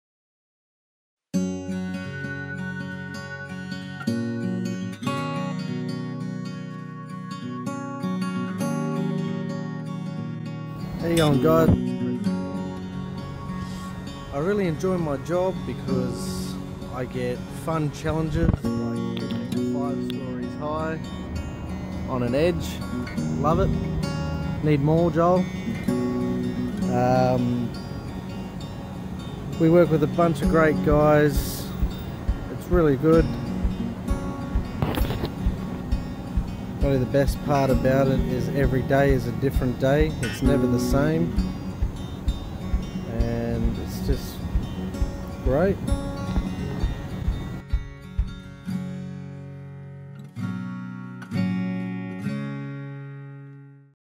Why i like my job in 1300 4 Gardening by Chris Campbell
1300 4 Gardening Chris Campbell creates a video on why he likes his job here at 1300 4 Gardening.